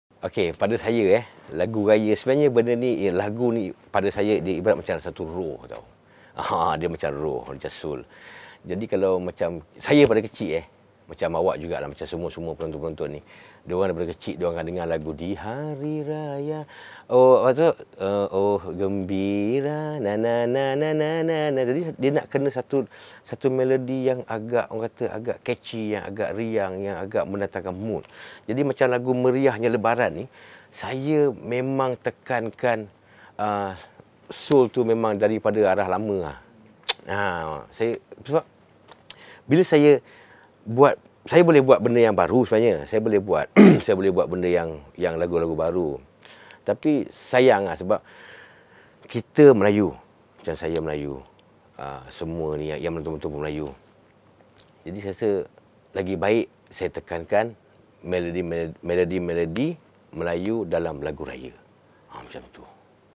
APAK HARRY KHALIFAH - LAGU ADA ROH??
Pandangan gitaris/komposer/penyanyi APAK/HARRY Khalifah mengenai Lagu Raya